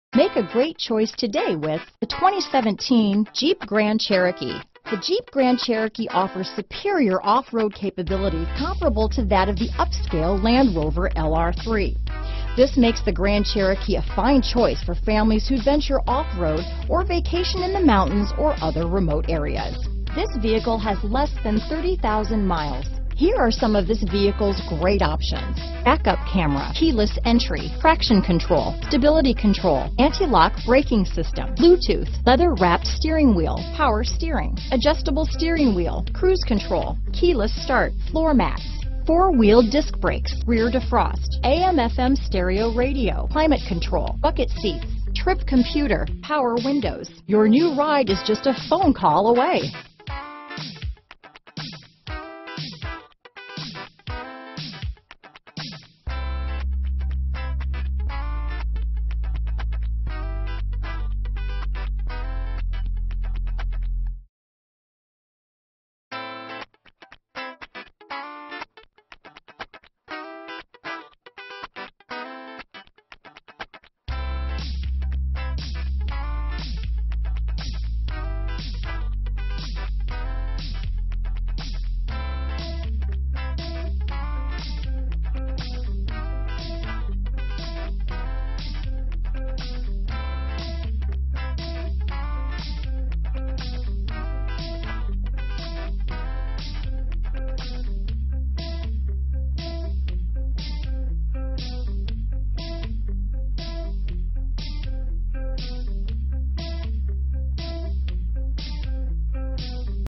2017 Jeep Grand_Cherokee Ontario, Los Angeles, Fontana, Glendora, Chino, CA 8160
Bright White Clearcoat Used 2017 Jeep Grand_Cherokee

4D Sport UtilityWhite Priced below KBB Fair Purchase Price! Altitude RWD 8-Speed Automatic 3.6L V6 24V VVTGrand Cherokee Jeep Odometer is 2925 miles below market average! 19/26 City/Highway MPGAwards:* 2017 KBB.com 10 Most Awarded Brands 2017